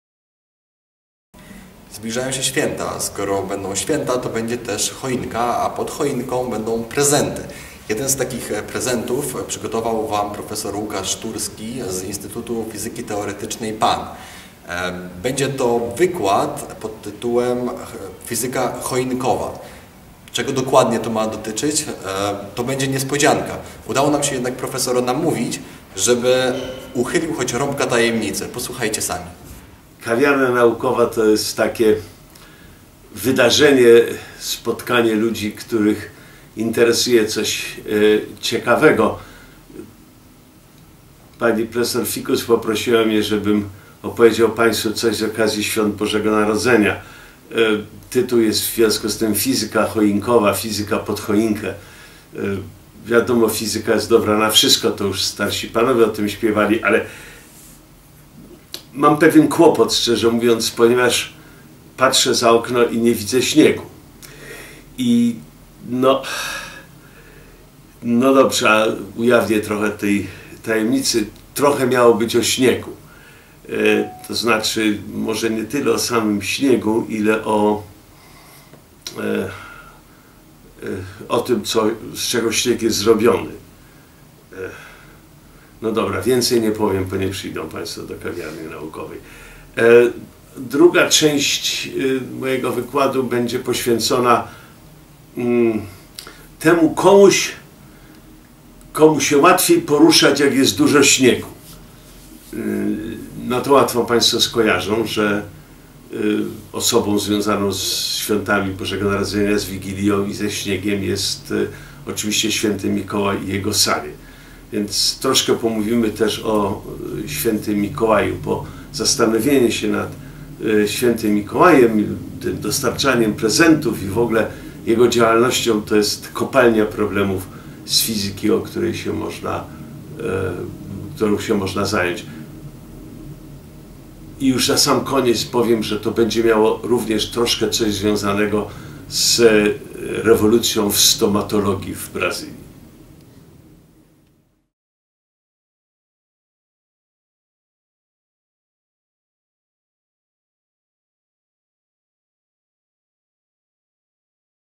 Zapowiedź: Prof. Turski - Fizyka choinkowa - 19.12.2011
Tytuł: Fizyka choinkowa i może nie tylko
Data: 19 grudnia 2011, godz. 18
Ekspert: Prof. Łukasz A. Turski (Centrum Fizyki Teoretycznej PAN)
Organizator: Kawiarnia Naukowa Festiwalu Nauki
Miejsce: Skwer Hoovera, Krakowskie Przedmieście 60a, Warszawa
Opis: W hipermarketach od dawna już słychać kolędy; niedługo pojawią się w naszych domach choinki. Ten okres zwielokrotnionych działań zakupowo-kuchenno-sprzątaniowych jest doskonałą okazją do odświeżenia trochę swoich wiadomości z. fizyki!

Nagranie powstało dzięki istnieniu europejskiego projektu SciCafe realizowanego przez grupę organizatorów Kawiarni Naukowych, finansowanego z 7. Programu Ramowego Komisji Europejskiej.